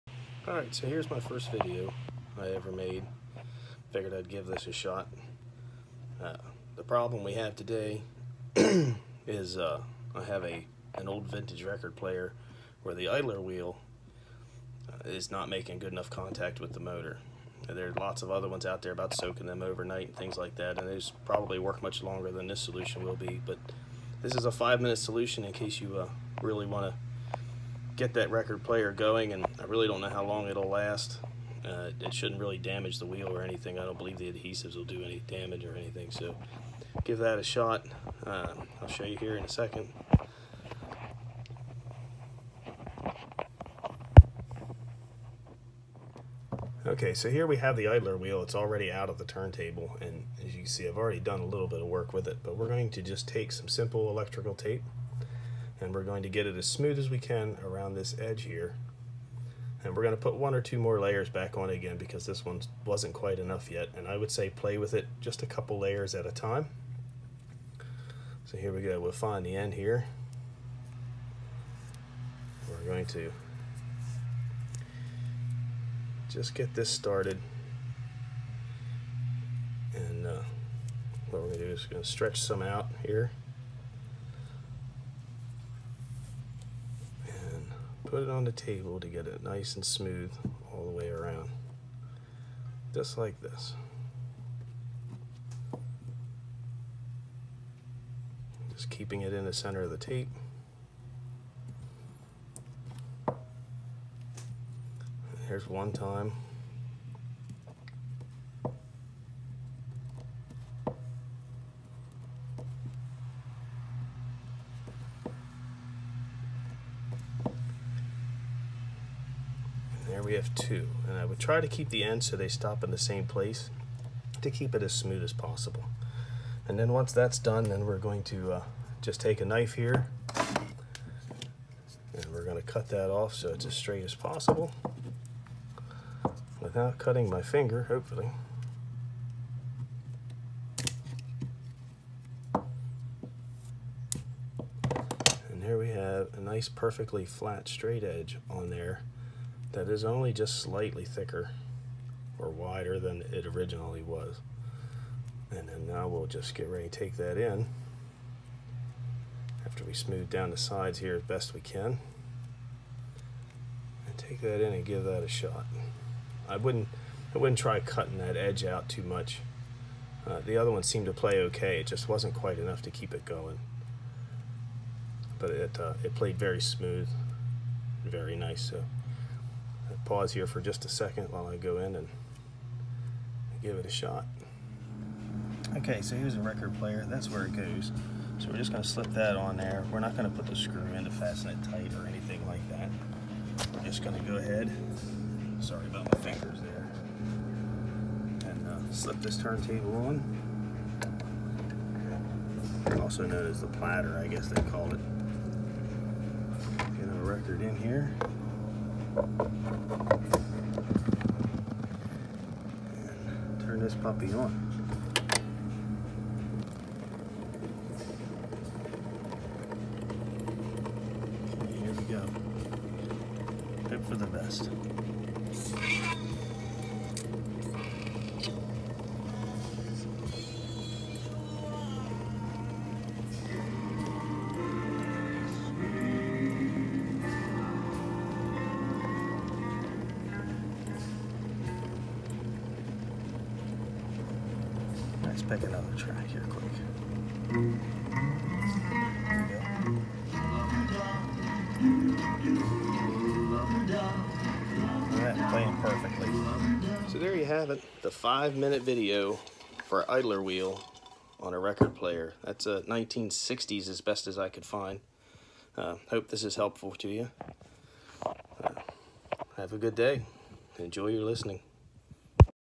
Five minute idler wheel fix from a vintage record player
Welcome to my maiden video. After several days of thinking of how to get my wife’s record player to work without having to use chemicals, spending much money, or having to wait on a purchase of a rebuilt one, I came up with this idea which seemed to work well. I haven’t played it long on the fix yet, so I have no idea how long it will hold up but it is cheap enough and takes so little time that I figure it wouldn’t matter much. Hope you enjoy.